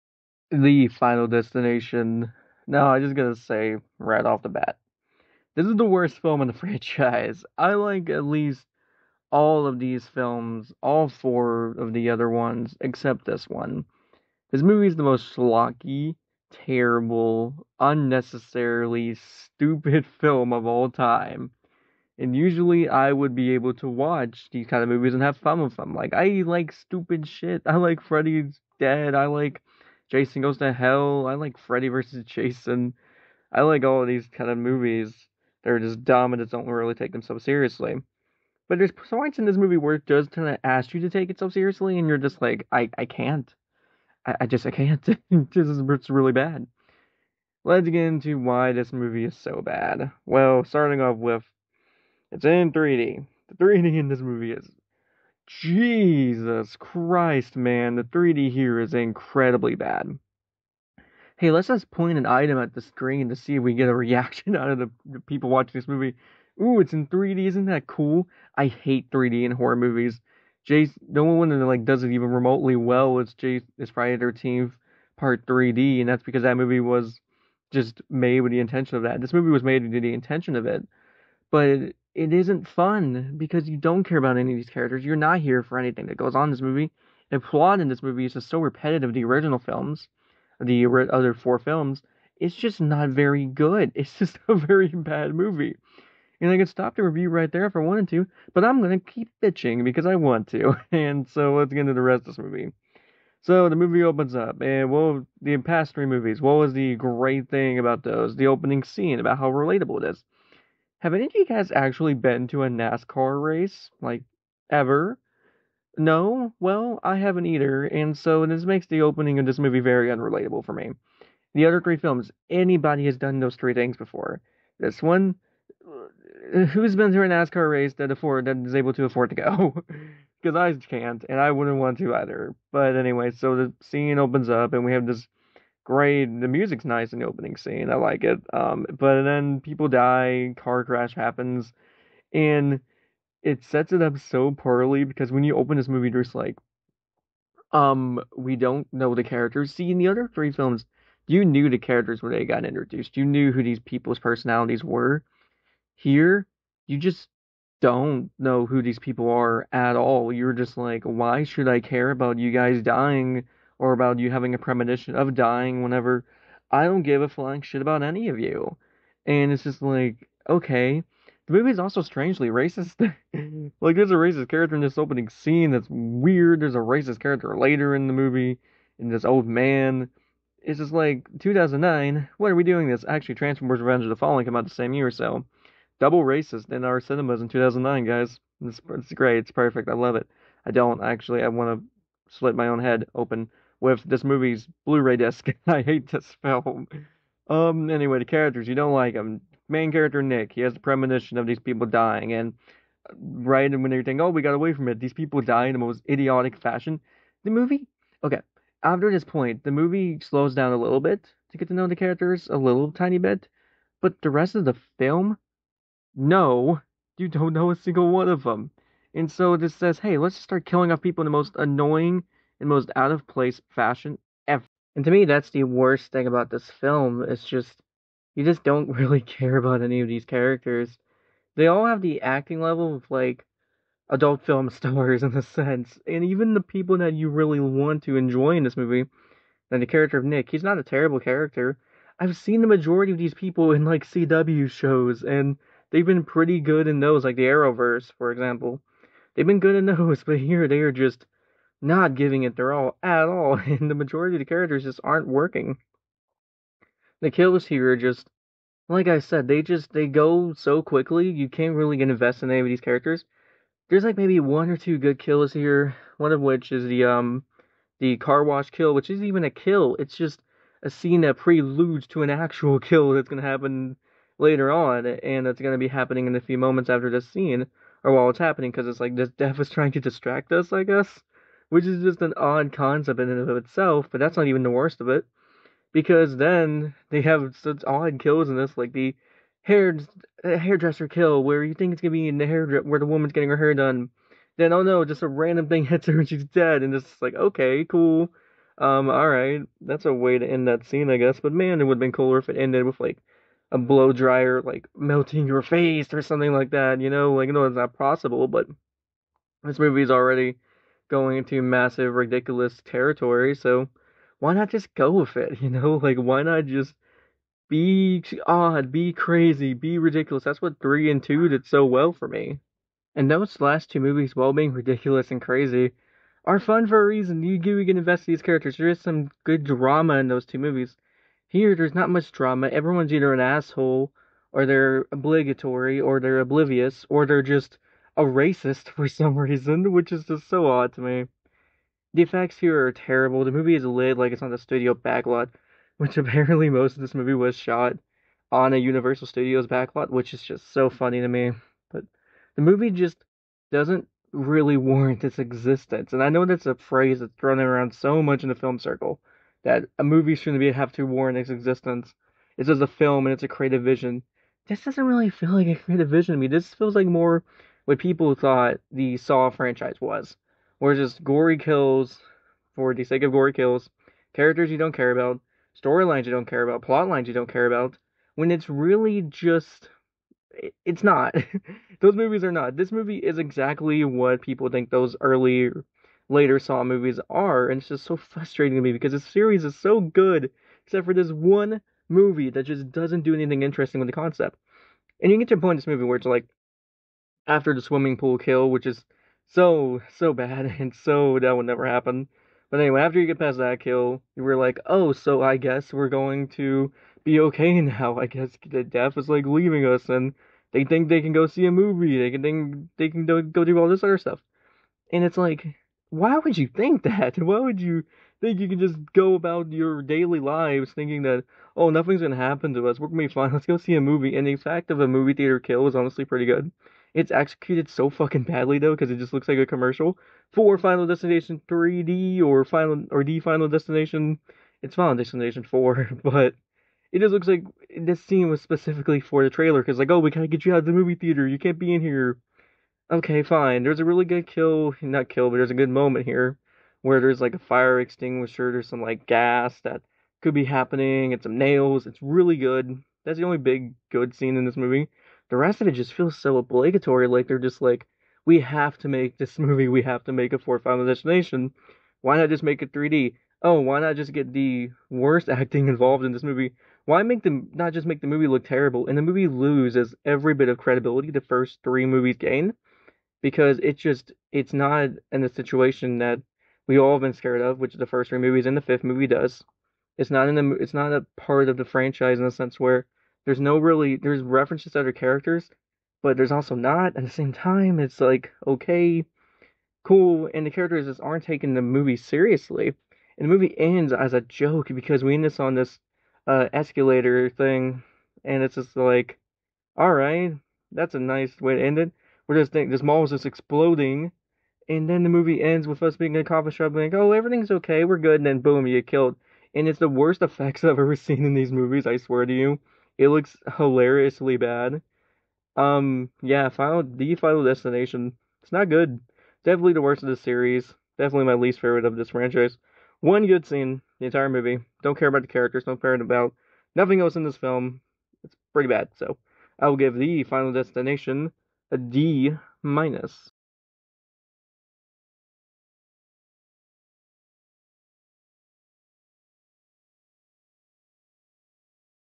The Final Destination (2009) - Movie Review
Let's talk about the 4th... and defiantly worst film in this franchise, The Final Destination